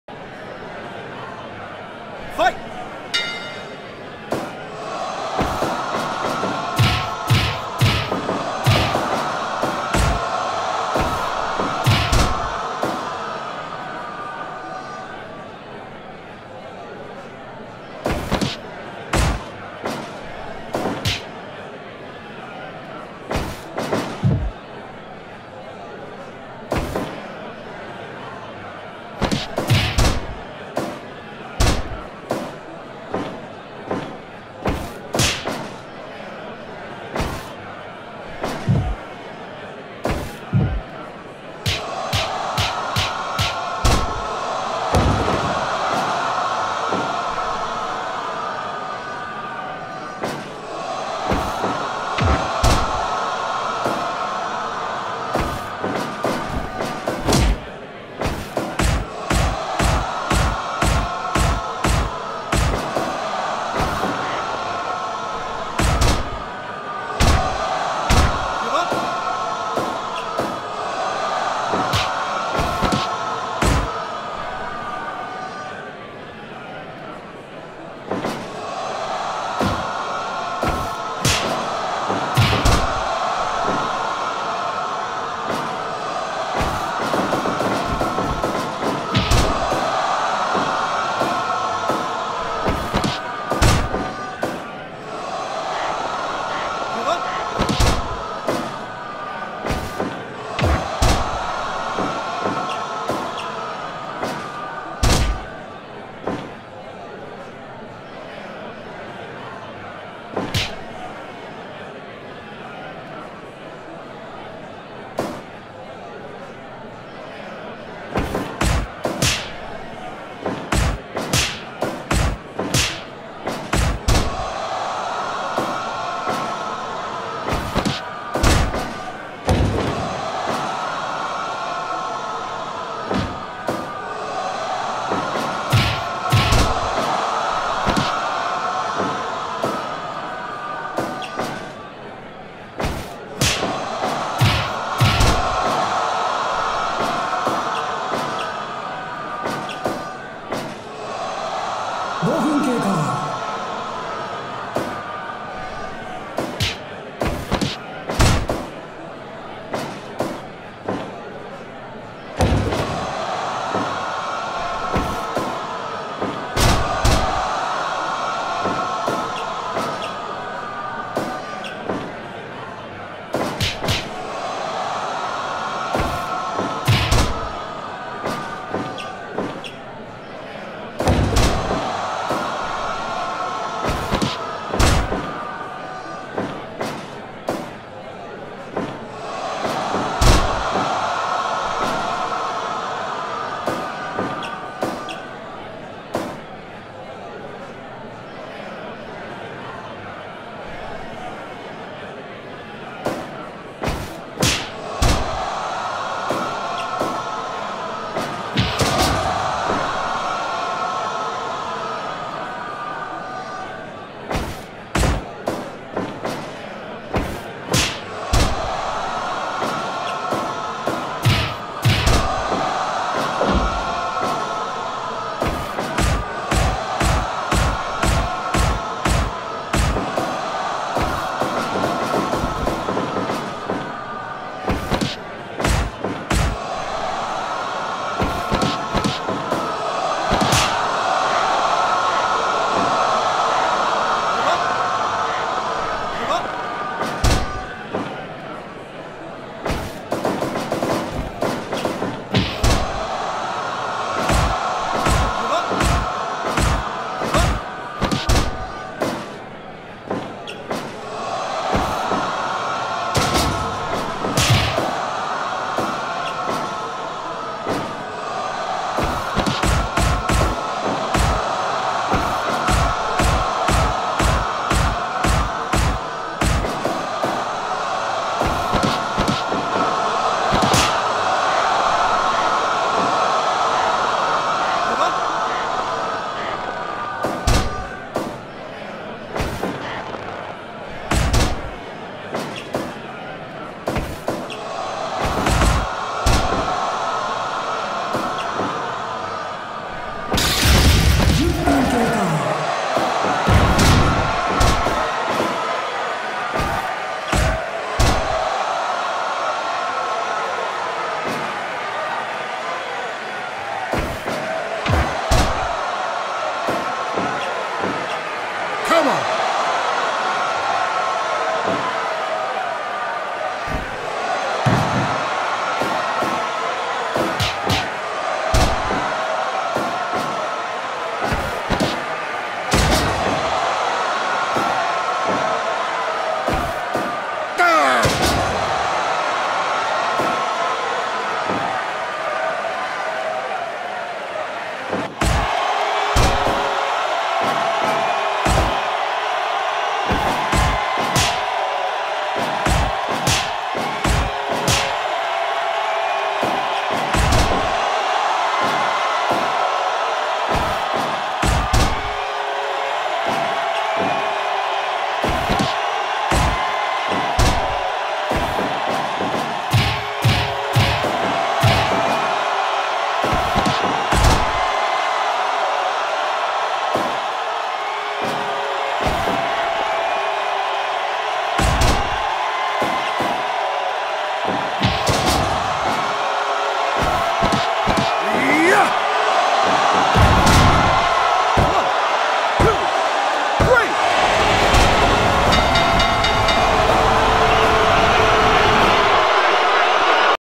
LOSER LEAVES TOWN 4 - MATCH #1,024 - JASON KANE VS TIKOKURA
LOSER LEAVES TOWN 4
QUARTER FINALS
MATCH #1,024 - JASON KANE VS TIKOKURA
HEAVYWEIGHT BOUT